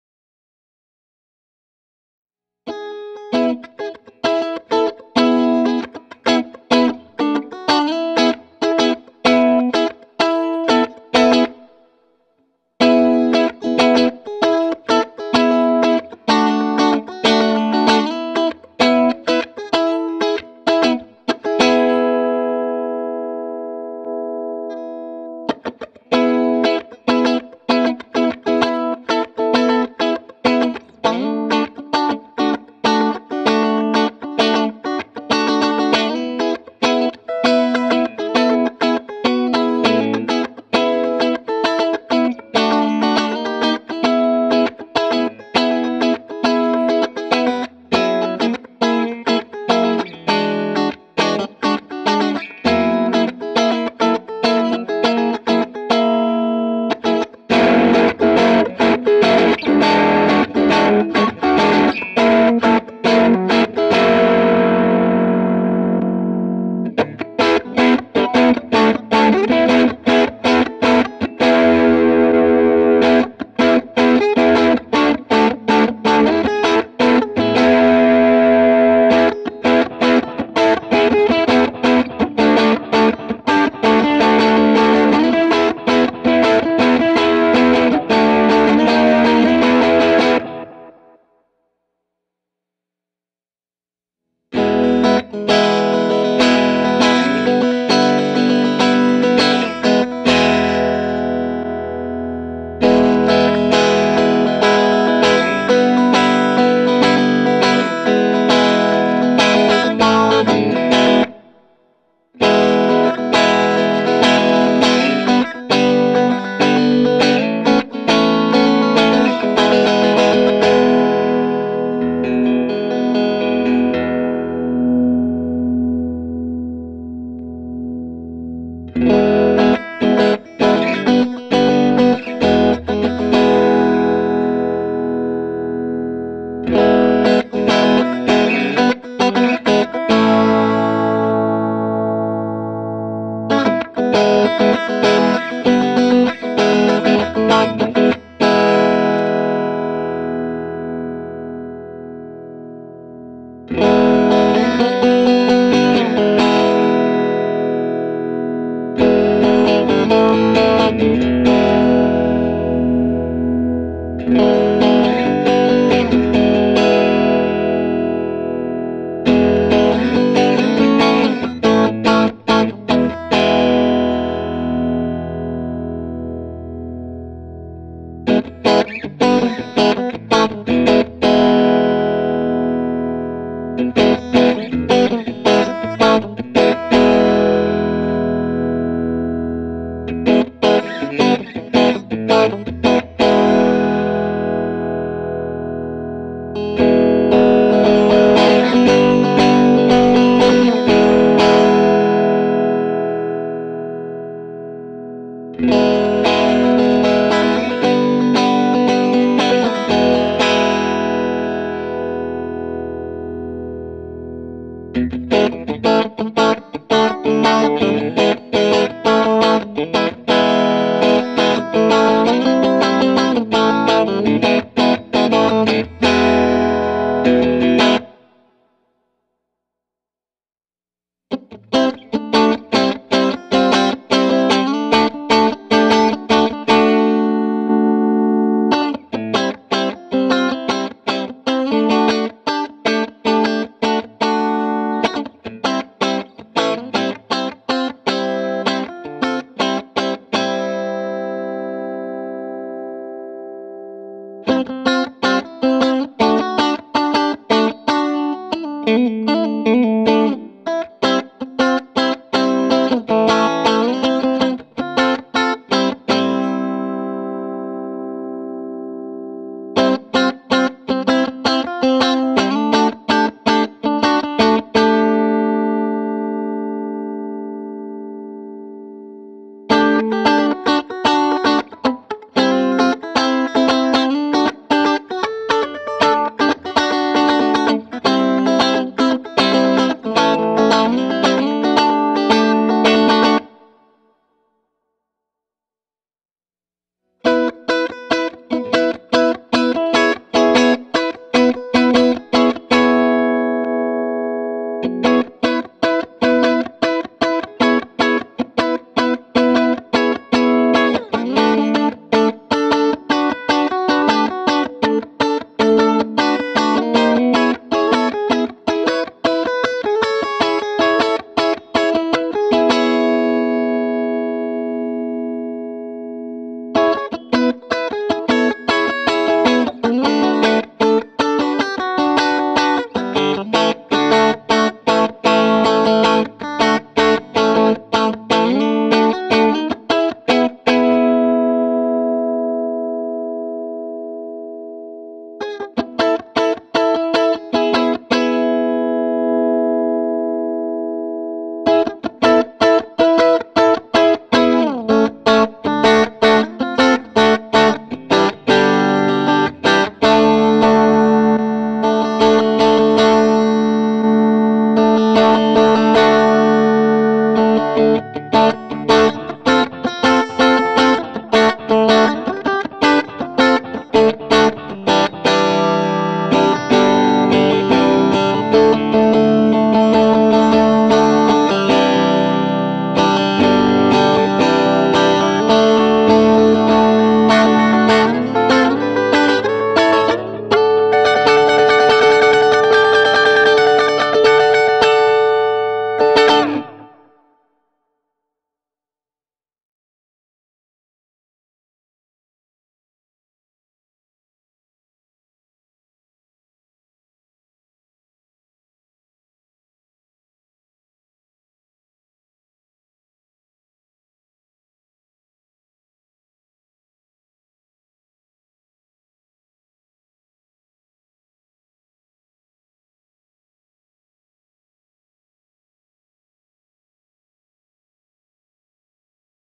Crazy Tube Circuits φ (Phi) Golden Ratio Compressor
The Crazy Tube Circuits φ (Phi) Golden Ratio Compressor has been updated to include 3 separate compressor circuits at a flip of a toggle switch.

An internal smart switching circuit lets you decide which type of compression works best for you featuring a wide range of compression ratio from 1:1 to 20:1

Each all-analog circuit is totally independent and has its own voice with different compression characteristics, range and response of controls to suit every style.

Gear used:

TMG Guitars Dover

29 Pedals EUNA
Thru-Tone PM-1
Chicago Stompworks Gray Speck

REVV's FX Loop:
Meris Enzo Synth
Strymon Zelzah Phaser
Chase Bliss Audio Tonal Recall Analog Delay